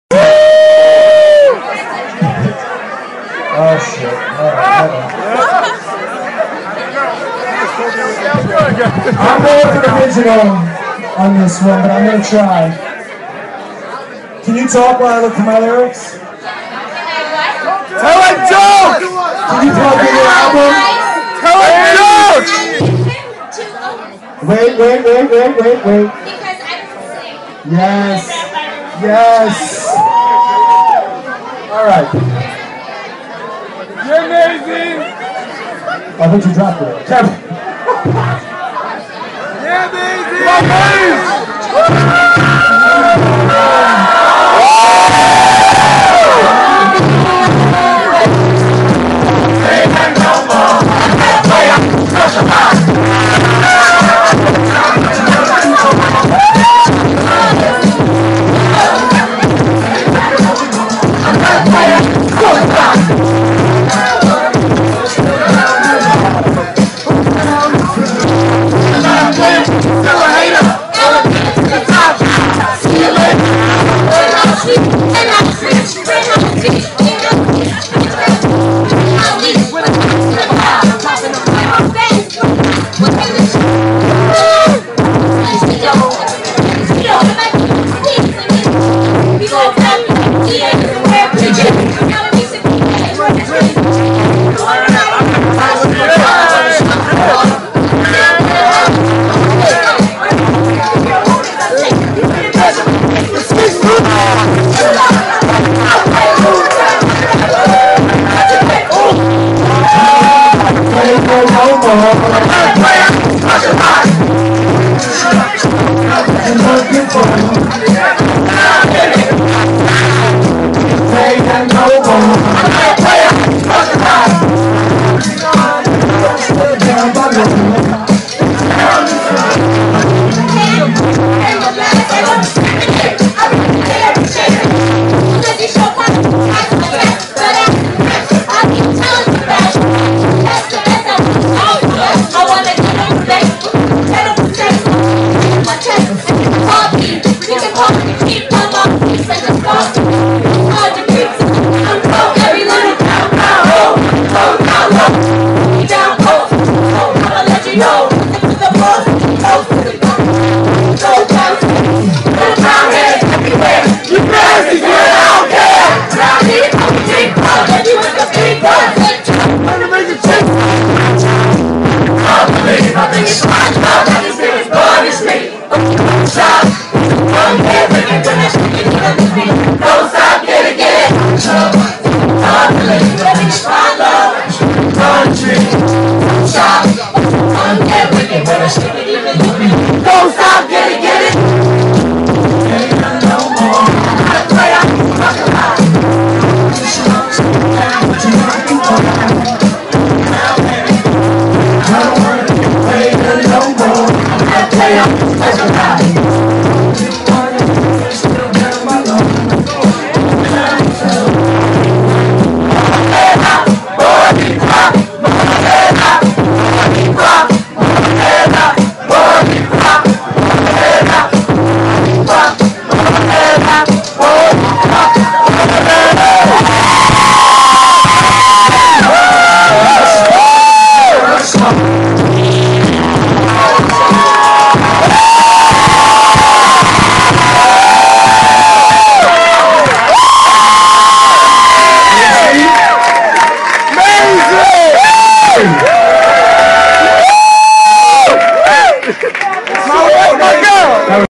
Hip Hop Karaoke ft. Maisie
Maisie kills it at Hip Hop karaoke, wins over fans across NYC.  Also, the sound is pretty terrible, but you get the idea.